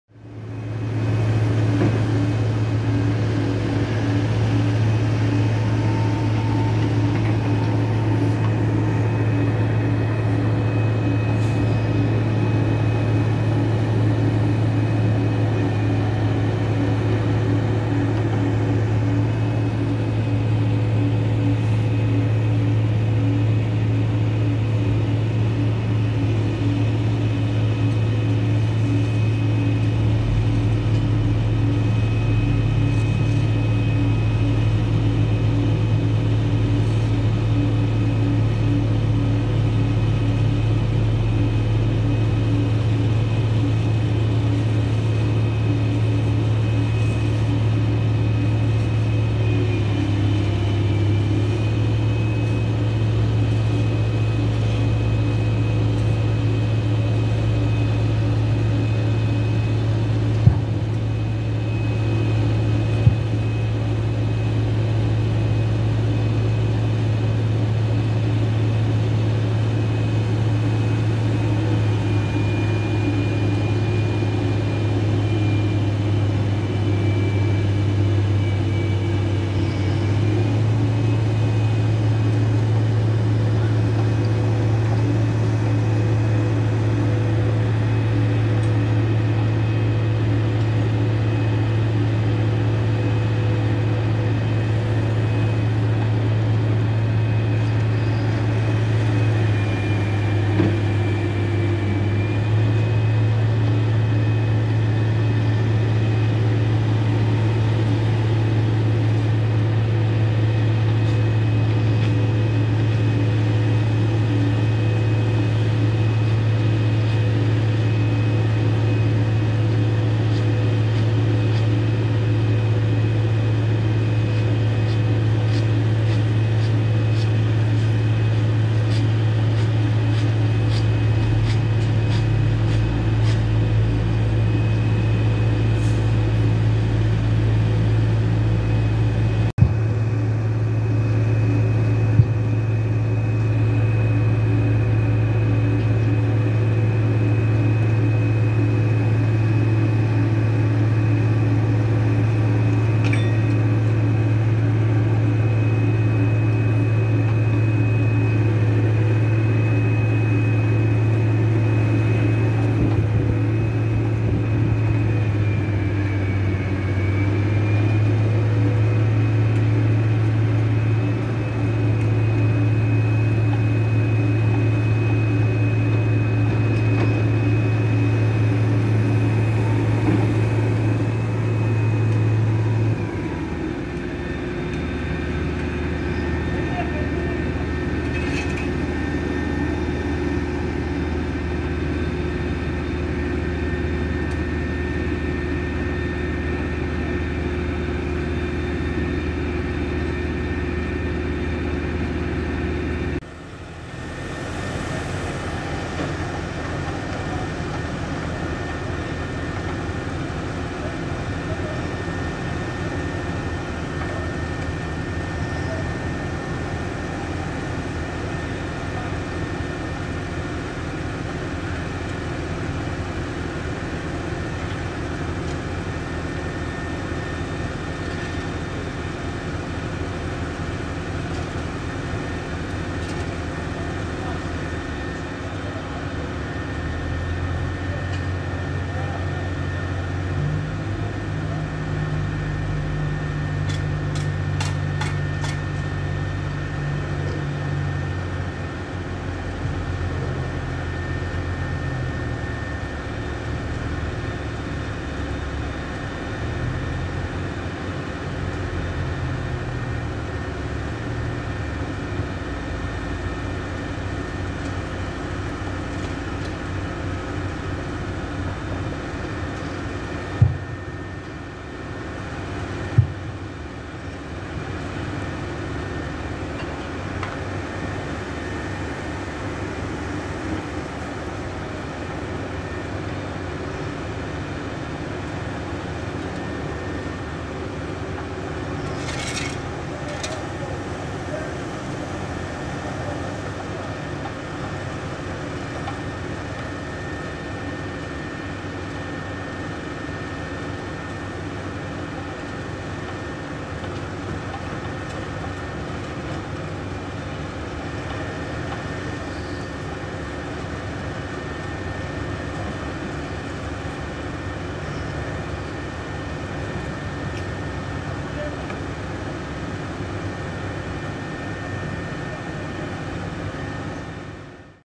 Керчане наблюдают за асфальтировкой двора по Индустриальному шоссе
Во дворе происходит что-то странное.